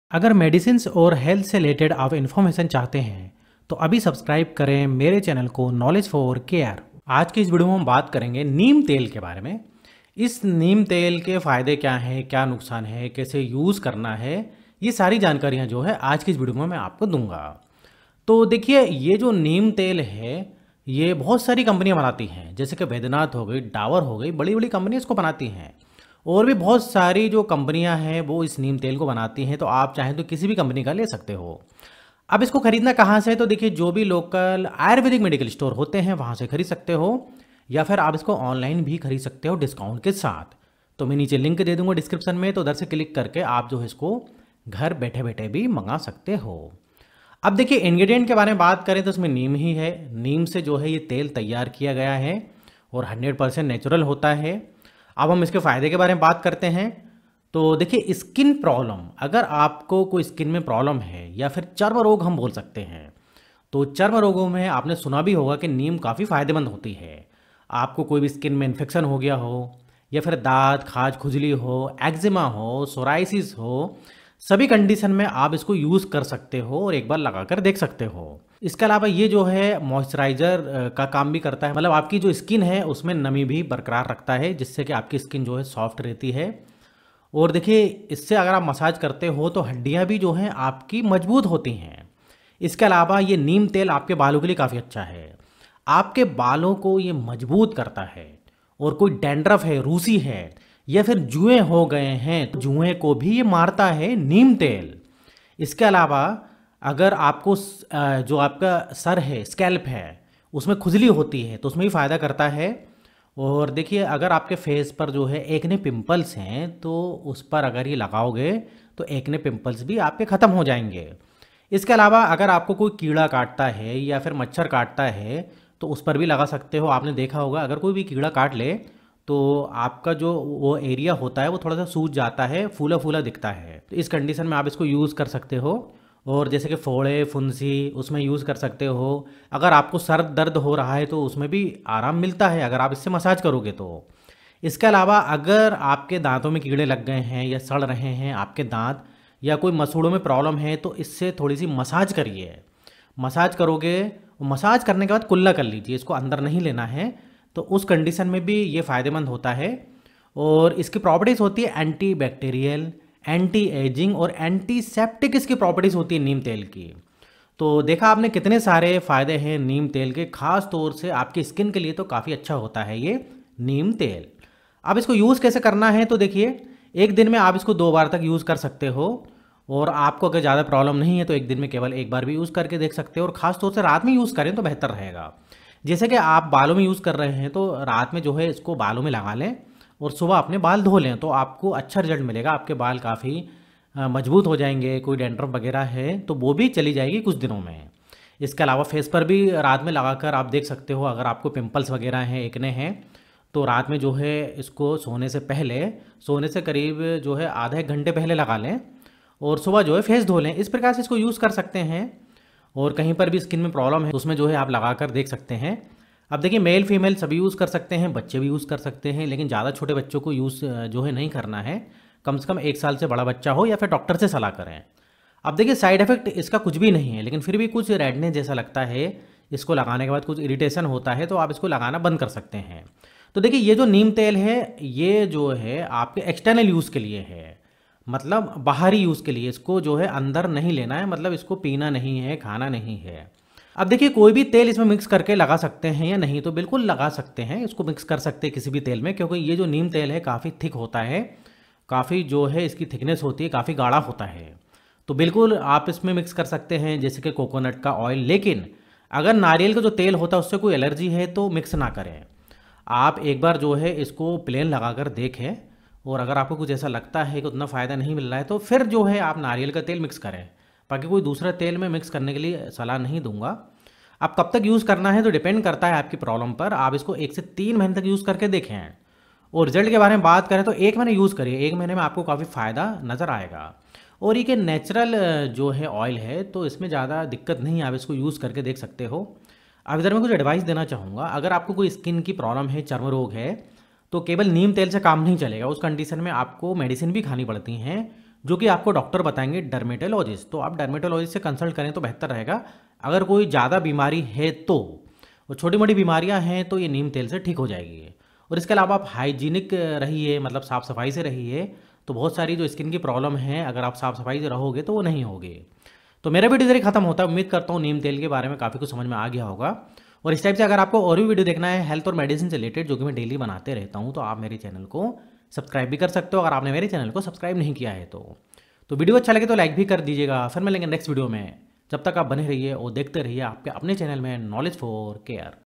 Neem Tel (Oil) Benefits, Dosage, Side Effects | Baidyanath, Dabur✅
Neem tel is your 100% natural solution to all skin problems.Neem tel strengthens the bones of kids, repairs the stress damage on skin for adults and provides moisture for aged skin. It has amazing anti-aging and antioxidant properties, which make Neem Tel the best solution for all skin issues.✅